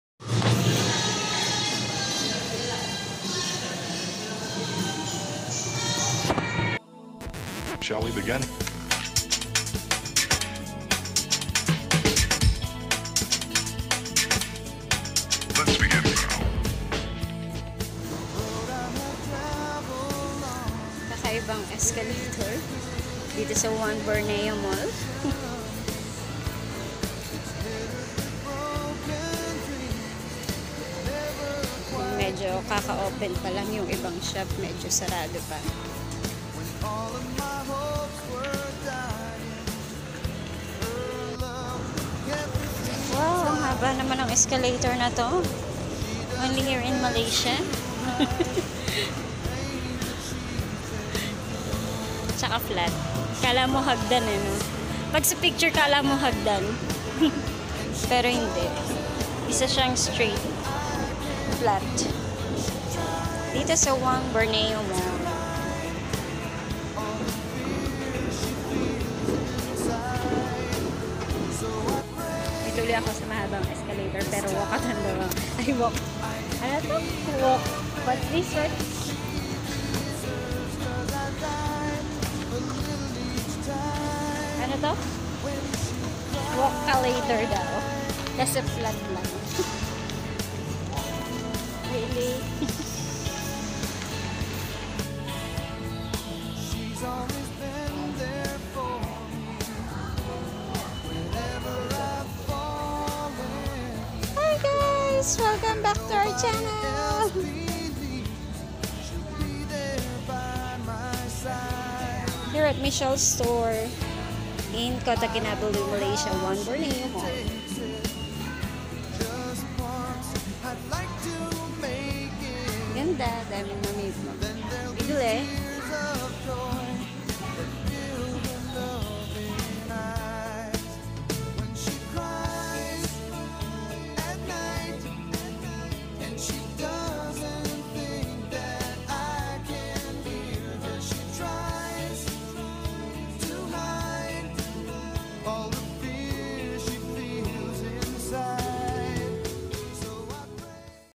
Vlog #7 - 1Borneo Hypermall Kota Kinabalu August 2019
Strolling the 1Borneo Hypermall Kota Kinabalu Malaysia 2019.